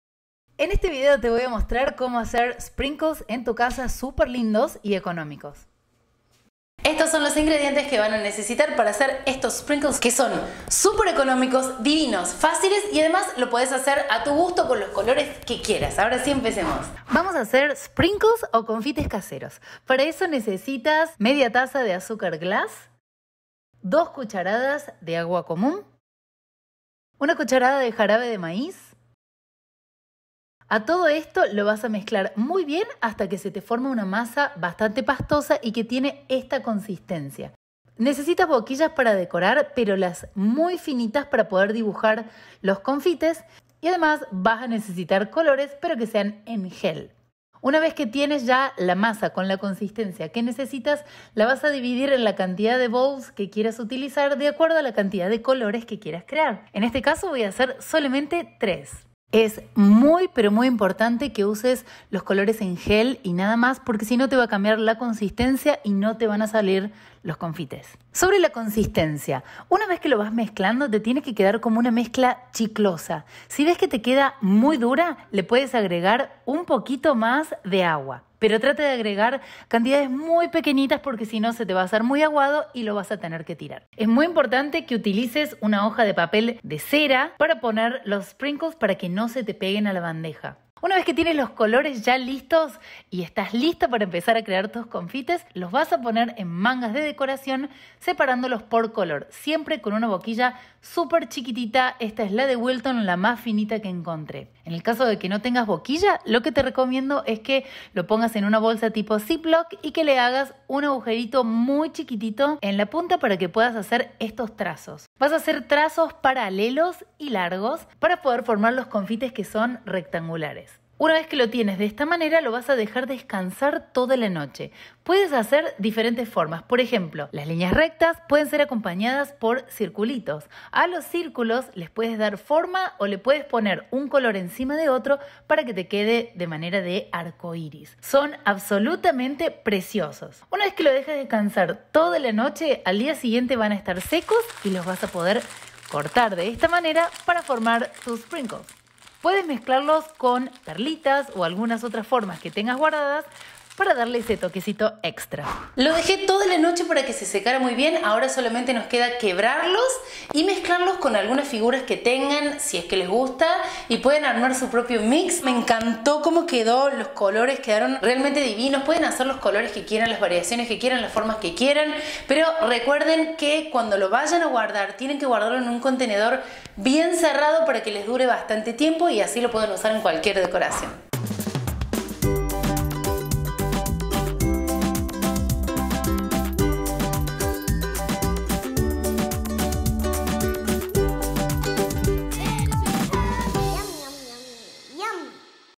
COMO HACER SPRINKLES CASEROS - GRAJEAS o CONFITES para pasteles. HOW TO MAKE SPRINKLES
Como hacer confites o sprinkles caseros, con solo 2 ingredientes! Paso a paso! Vas a amar esta receta tan facil.

Quieres aprender recetas faciles y la receta de buittercream que no falla! Ven a mi nuevo curso de reposteria! Buttercreams infalibles!